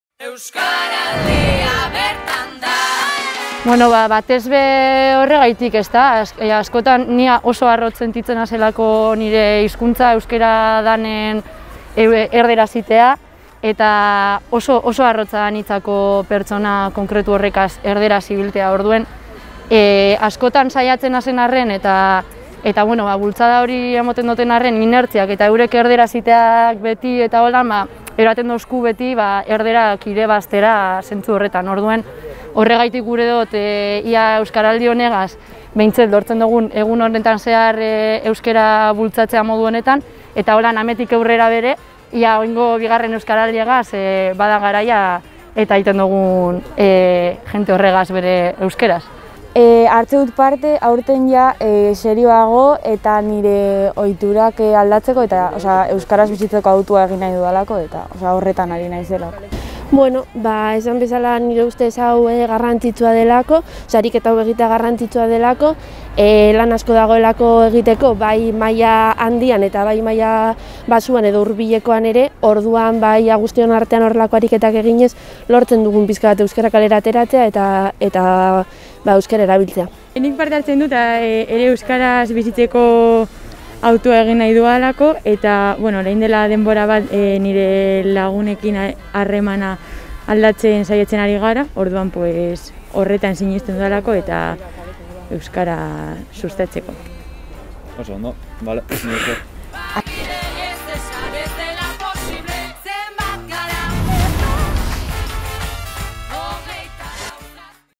Ongi etorri Plentziako Euskaraldira!
Plentziako zenbait eragilek eta herritarrek Euskaraldiari atxikimendua adierazi diote, hona hemen batsuen hitzak.
Euskaraldiaren helburu nagusia herritarren hizkuntza ohiturak aldatuta euskararen erabilera handitzea da. 2020an, azaroaren 20tik abenduaren 4ra egingo dugu ariketa.